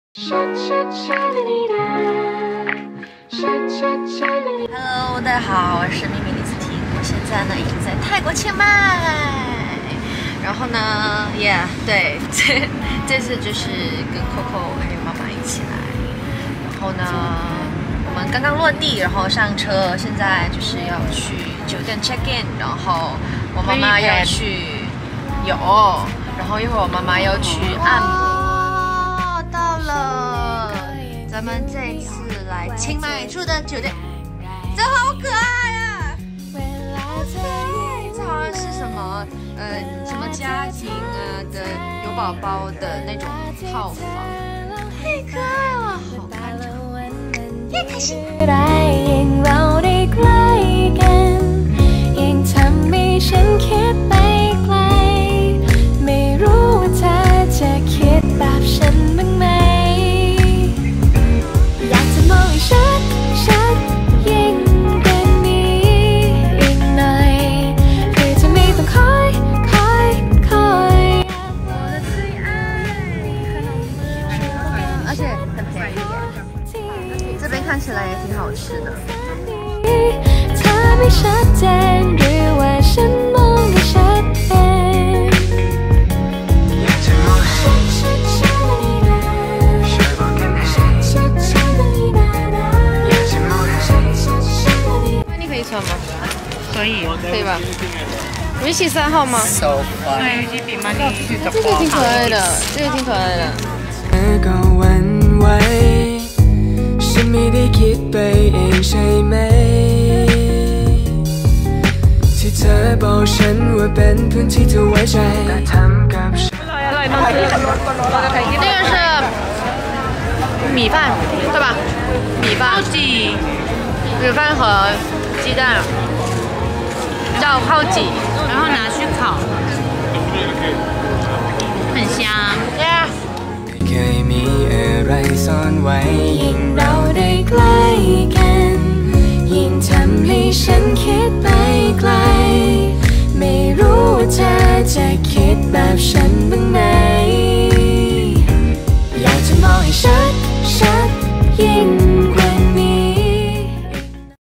李紫婷 Mimi Lee 回泰国啦【泰国清迈】VLOG EP.01
230320 微博更新

今天打开任意门，第二站「 清迈 」☀️✈️